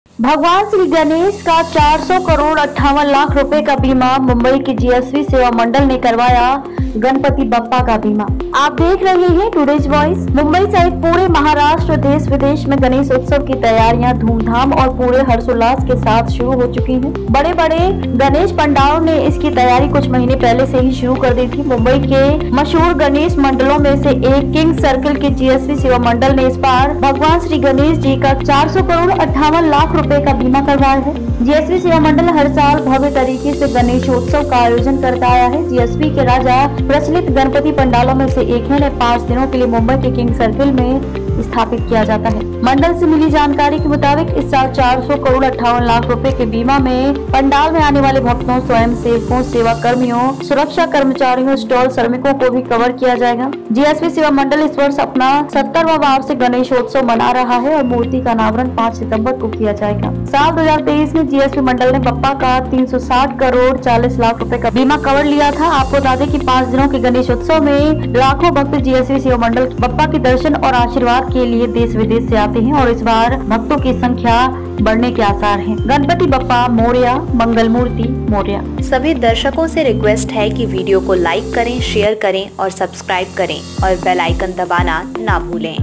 Insurance of Rs 400.58 crore for Lord Ganesh! GSB Seva Mandal of Mumbai got Ganpati Bappa insured
Insurance of Rs 400.58 crore for Lord Shri Ganesh! GSB Seva Mandal of Mumbai got Ganpati Bappa insured.

Kings Circle (Mumbai)

Preparations for Ganesh Utsav have started with pomp and gaiety in Mumbai and across Maharashtra, the country and abroad.

Big Ganesh pandals had started its preparations a few months ago. GSB Seva Mandal of Kings Circle, one of the famous Ganesh Mandals of Mumbai, has got Lord Shri Ganesh Ji insured for Rs 400.58 crore this time.

GSB Seva Mandal has been organizing Ganeshotsav in a grand manner every year. GSB's Raja is one of the popular Ganpati pandals. They are installed in King Circle, Mumbai for 5 days.

According to the information received from the Mandal, this year the devotees, volunteers, service personnel, security staff, stall workers coming to the pandal will also be covered in the insurance of Rs 400.58 crore.

 GSB Seva Mandal is celebrating its 70th annual Ganeshotsav this year and the idol will be unveiled on September 5.

In the year 2023, GSB Mandal had taken an insurance cover of Rs 360.40 crore for Bappa.

Let us tell you that during the 5-day Ganesh festival, lakhs of devotees come to the country and abroad for the darshan and blessings of Bappa of GSB Seva Mandal and this time the number of devotees is expected to increase.

Ganpati Bappa Morya, Mangalmoorti Morya